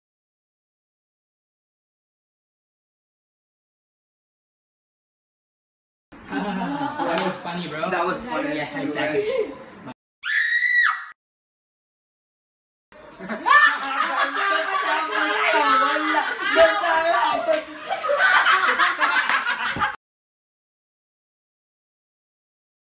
How they laugh ! White people vs brown
A collaboration between Maaz and Sufiyan !
In this video you will see something  which you all must have experienced at least once in your life !
This is a video about how white people (Educated ones) vs brown people (illiterate ones)